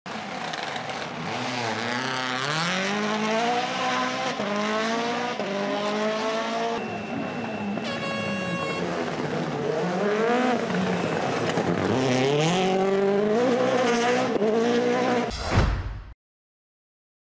Neuville Quickest on SS1 | WRC Secto Rally Finland 2022
Slidin' to a stage win on Rally Finland - Thierry Neuville/Martijn Wydaeghe 😎